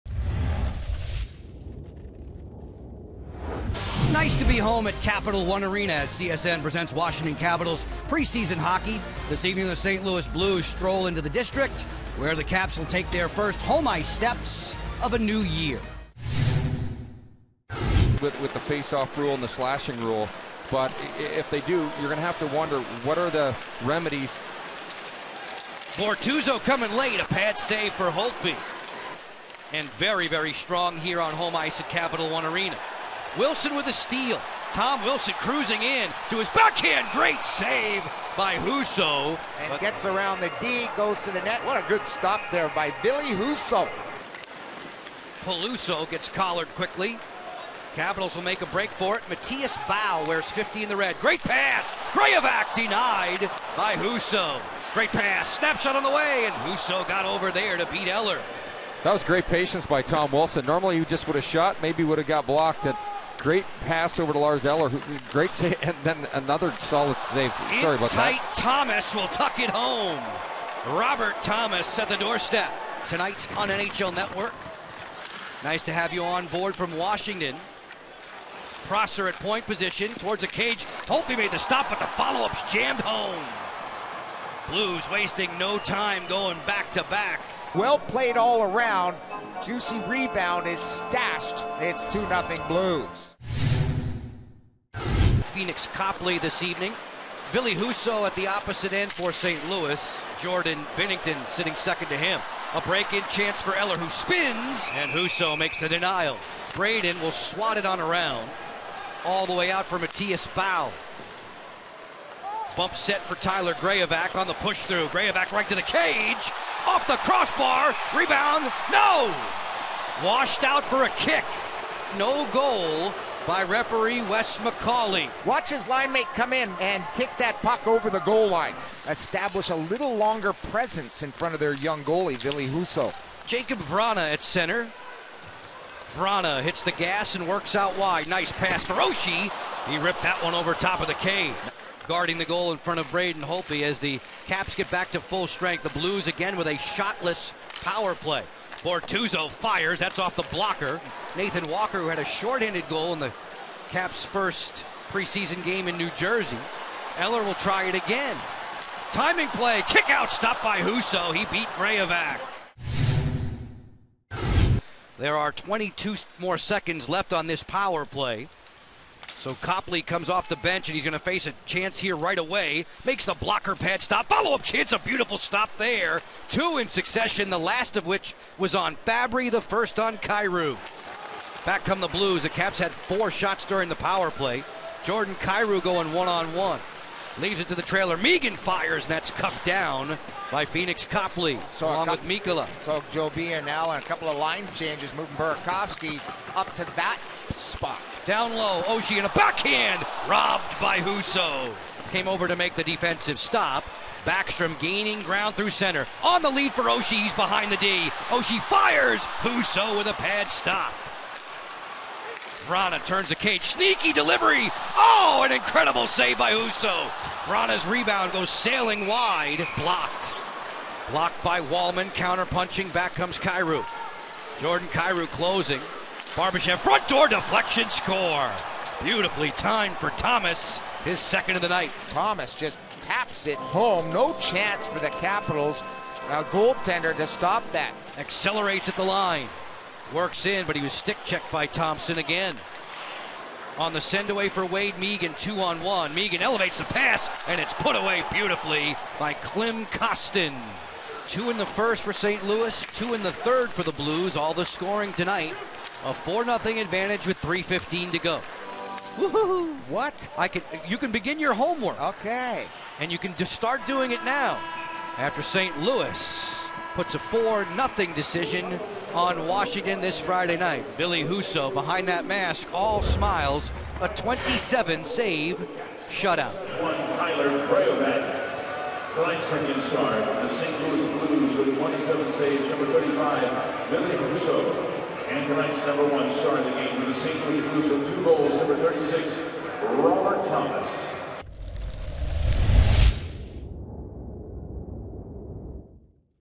St. Louis Blues vs Washington Capitals - September 22, 2017 | Game Highlights | NHL 2017/18
2017/18 - (Pre Season)